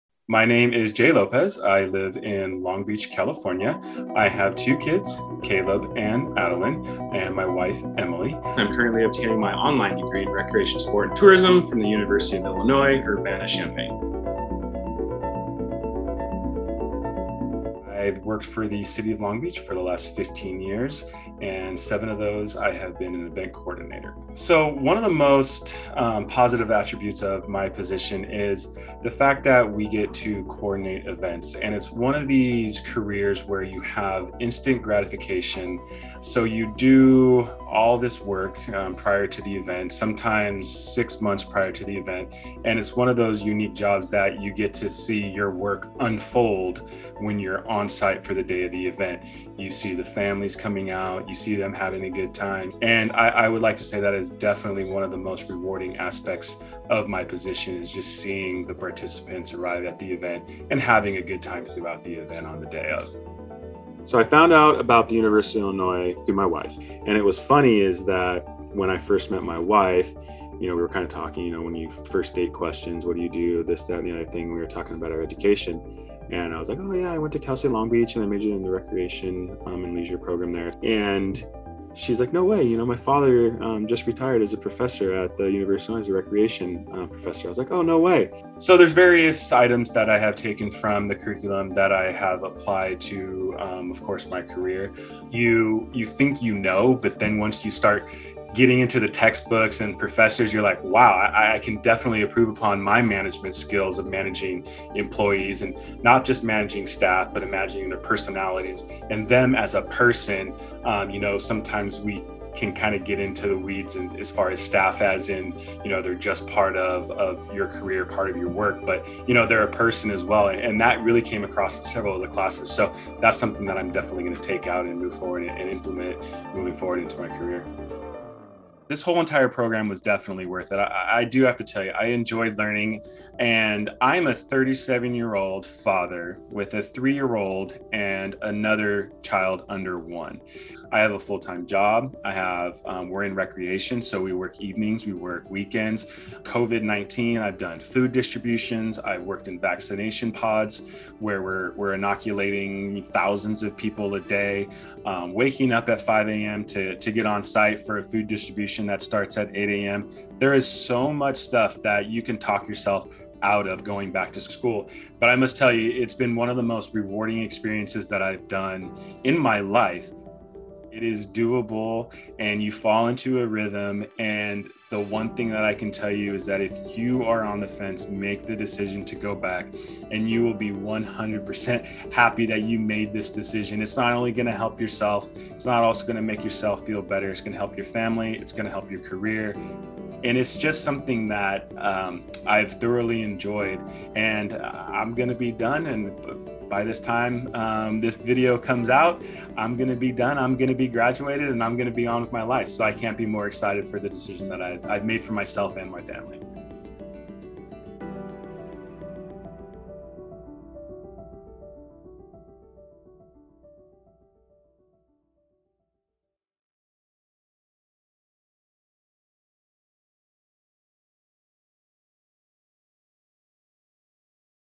One Story: UIUC Online Master’s Student, Jay Lopez, Recreation, Sport & Tourism
Meet Jay Lopez, a University of Illinois Urbana-Champaign online master’s student in Recreation, Sport & Tourism.

Jay lives in Long Beach, California, with his wife, Emily, and their two young children. He works for the city as an events coordinator and found out about UIUC through Emily. Jay quickly realized how helpful this degree would be for his management skills and immediately started using them while running COVID-19 food distribution and vaccination pods. Jay wants people to know that pursuing an online graduate degree is doable while working full-time with a young family. And that this experience has been one of the most rewarding in his life.

We hope that you enjoy Jay’s story and that it gives you an inside look at graduate college at UIUC! Want to learn more about UIUC graduate education?